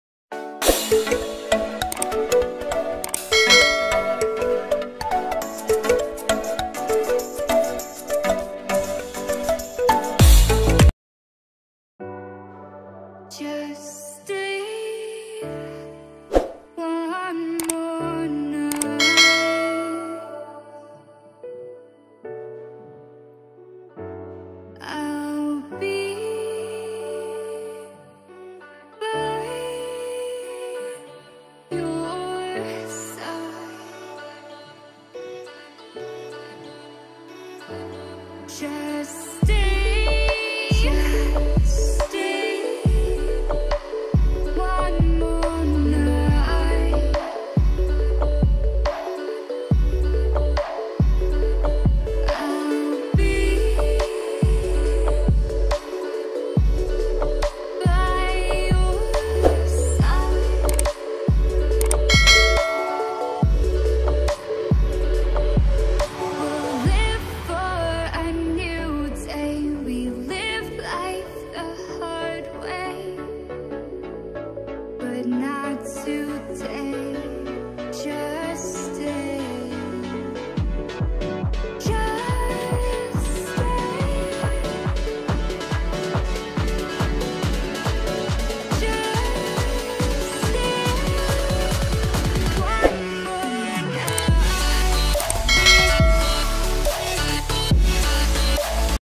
Easy and Quick Barbie Cake Decoration For Beginners l Doll Cake At Home
Most Satisfying Cake Decorating Ideas, Beautiful Homemade Cakes Yummy Cakes Tutorial, Chocolate Cakes Recipe and More...